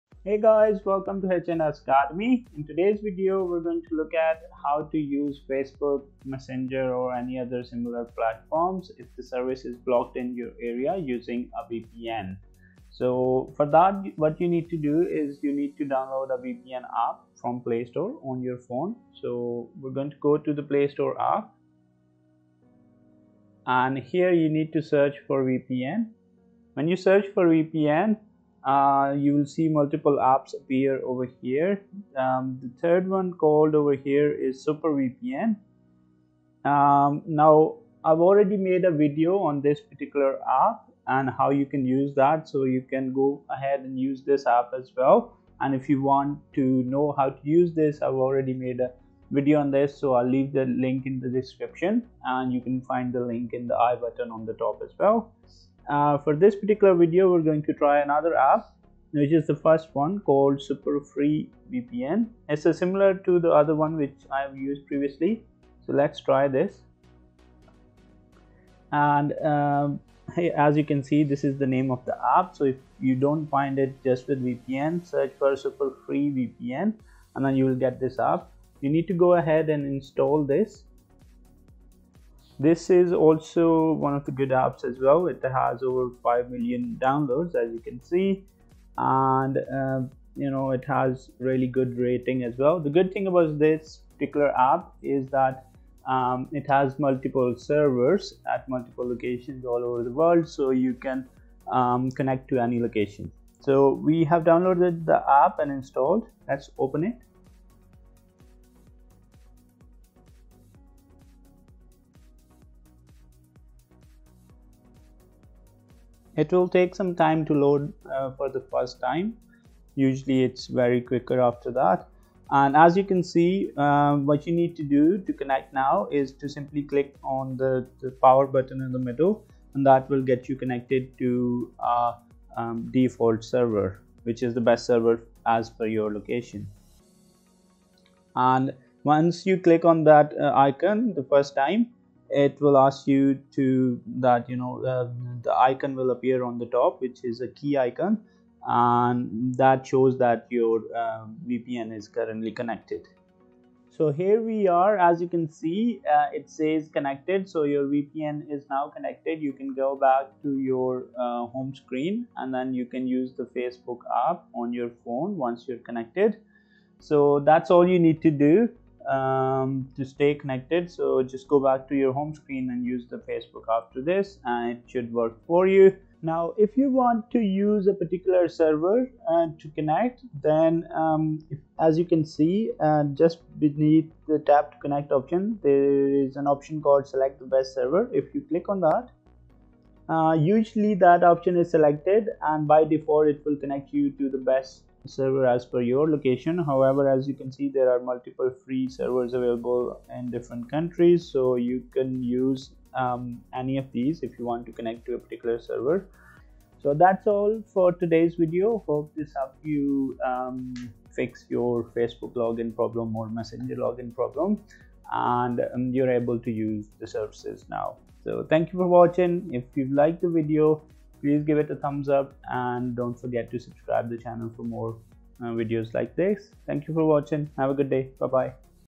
VPN for Facebook | Facebook Login Problem | Super Free VPN App
Here's a quick tutorial on how to use VPN for Facebook and get rid of the Facebook login problem. I have used the Super Free VPN app for this tutorial which has a 4.5-star rating and over 5 million downloads on the play store.

Recently there's a ban on Facebook/messenger apps in Bangladesh so this video will help you solve the Facebook login problem and will help you learn how to use Facebook with VPN.

Super Free VPN App is a free tool that is easy to use with no registration required, has multiple servers, and one-click connect if you just want to use the default server. So go ahead and enjoy this free VPN for facebook/messenger etc.